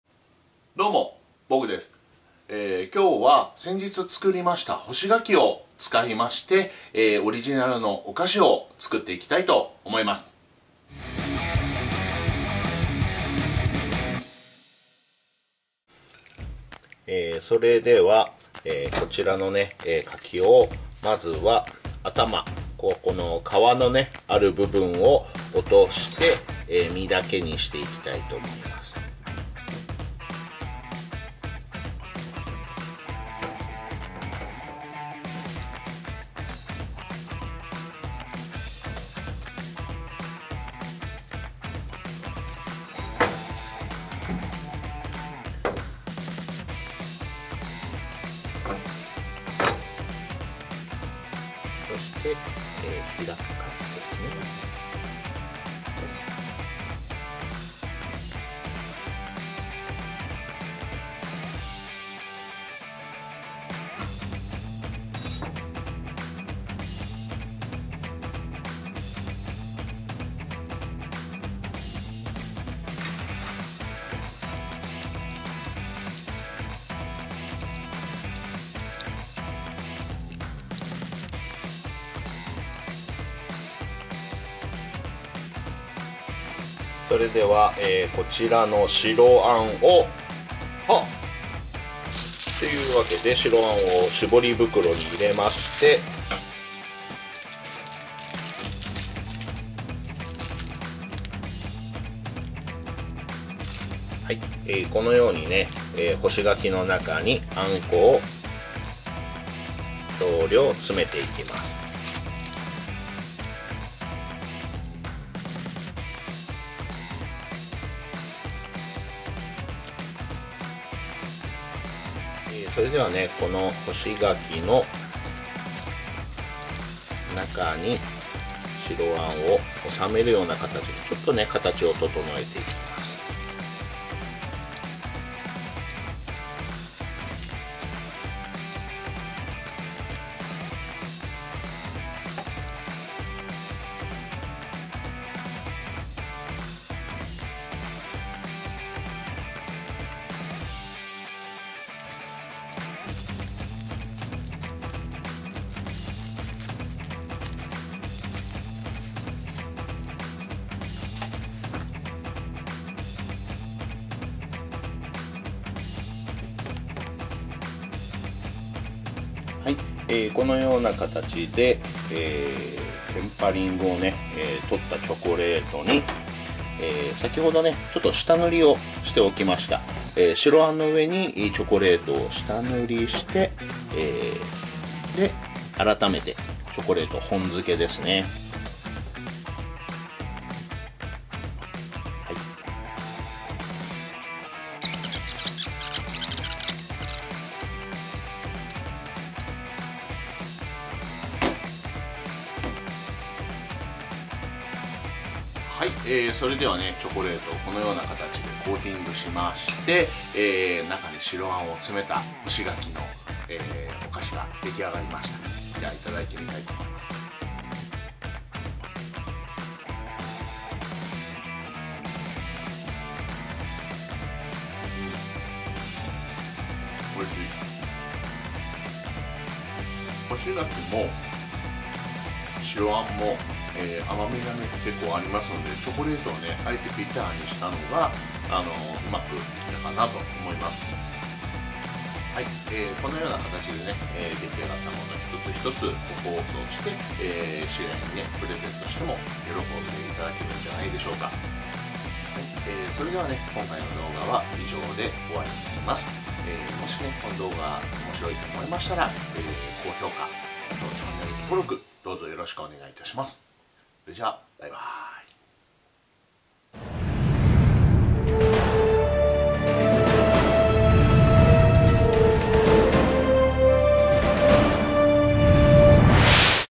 自家製干し柿で簡単スイーツ作り！ ｂｙタルトタタン
その昔、お砂糖が無い時代から食べられていた干し柿は当時にしては貴重な甘味だったそうです。
そんな干し柿を使ってお手軽簡単スイーツを作ってみました。

ｂｙタルトタタン

撮影　：　Sony HDR-AS100v
編集　：　iMovie(iPhone6s)
出演　：　タルトタタン
HDR-AS100vでファインダー無し撮影（手持ち）
カテゴリ　ブログ
ライセンス　標準のYouTubeライセンス